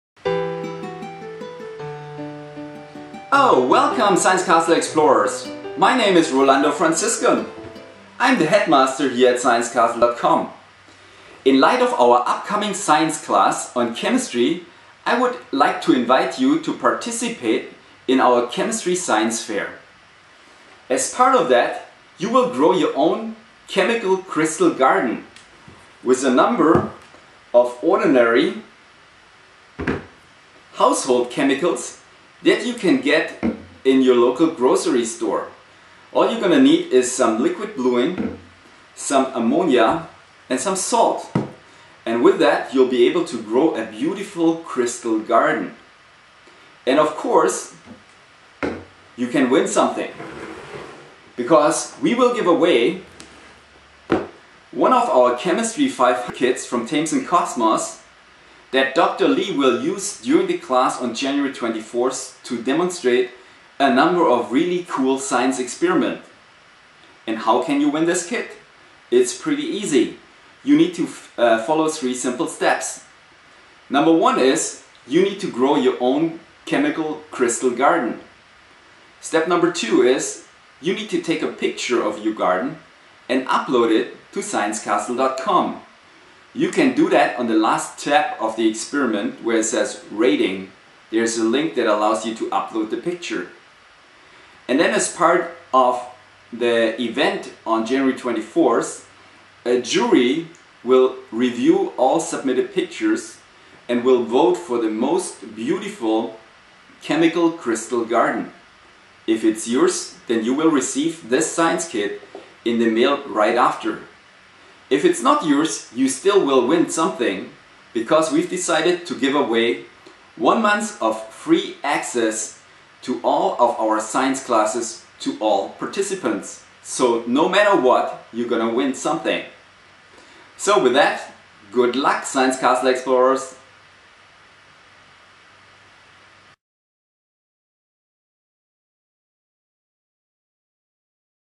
Crystal Garden Science Fair - Win a Chemistry Kit
- In light of our upcoming chemistry class we would like to invite you to participate in a fun little science fair. Grow the most beautiful crystal garden and win a chemistry science kit!